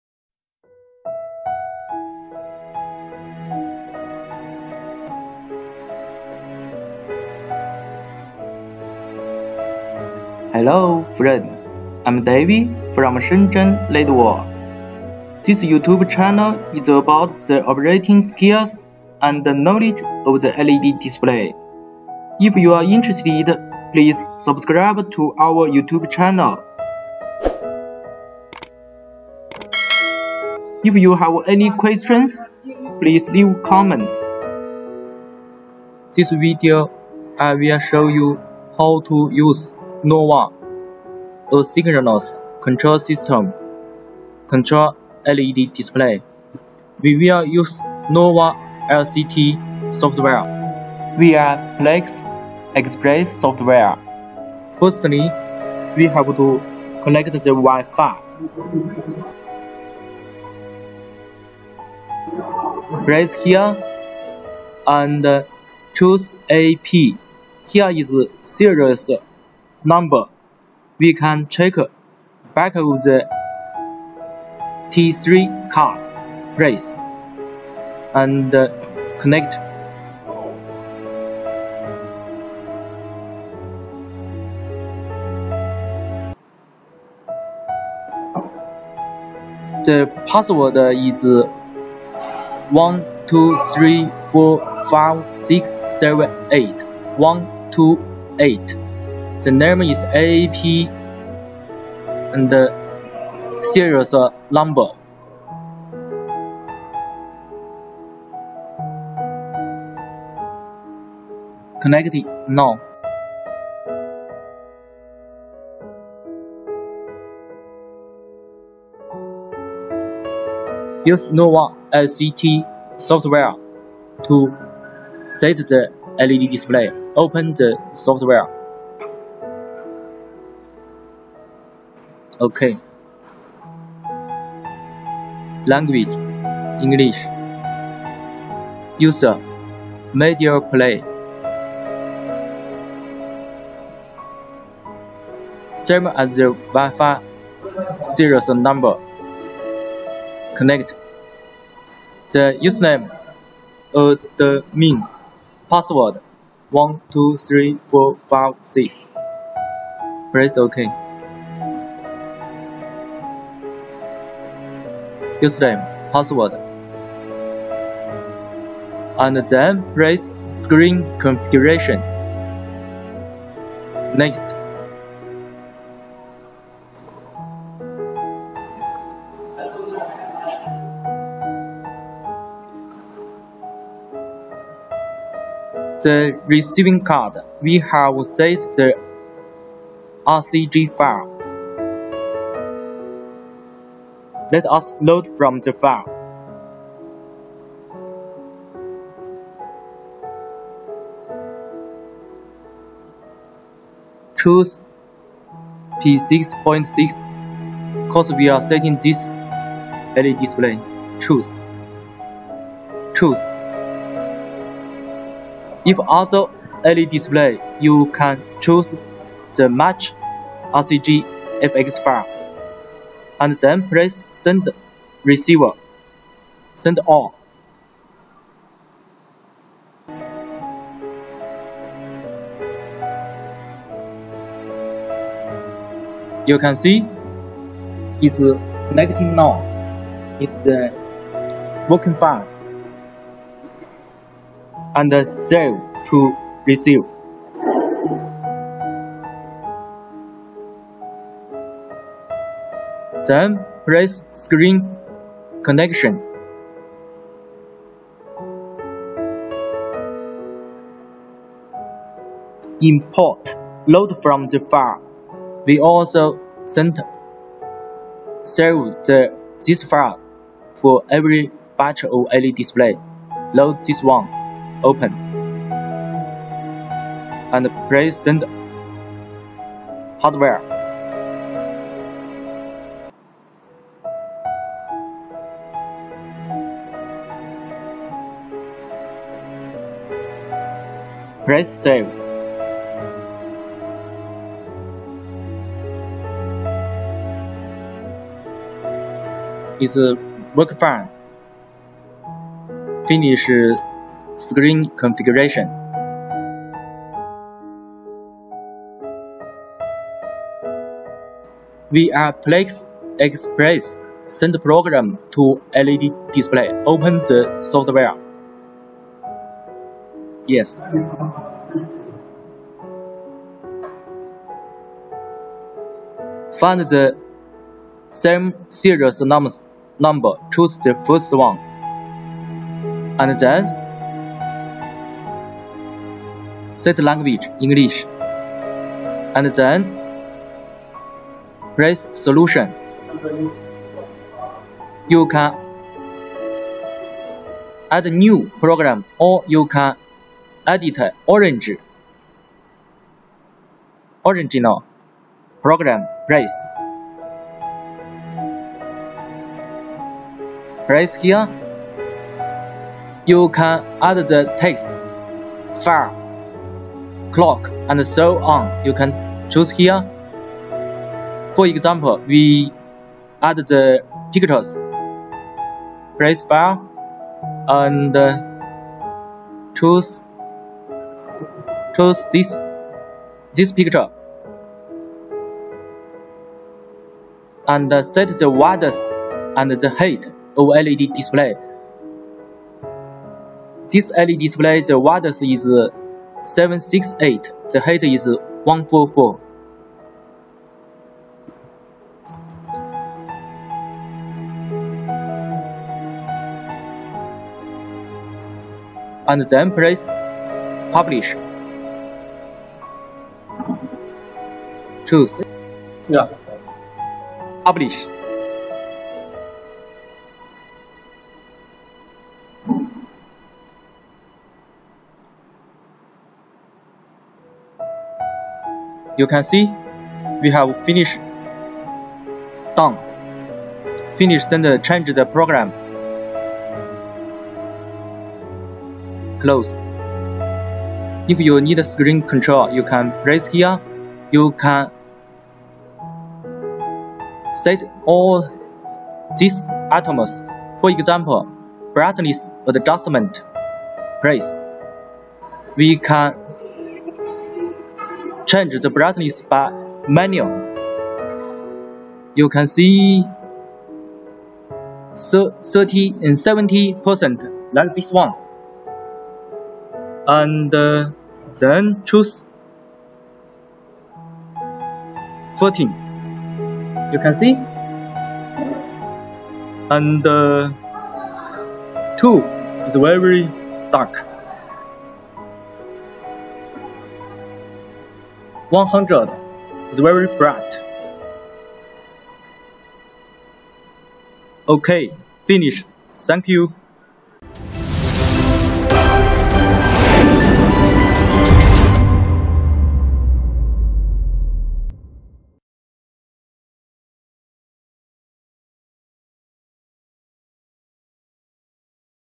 Asynchronous control system LED display setup, Newest Video tutorial - 3 steps to know Novastar set
There are only 3 steps, and you will know how to operate the asynchronous control system of Novastar.
How to connect wifi?
How to set NovaLCT?
How to set up Viplex Express software?
How to control LED displays?
How to send programs...

@Shenzhen LED World Co., LTD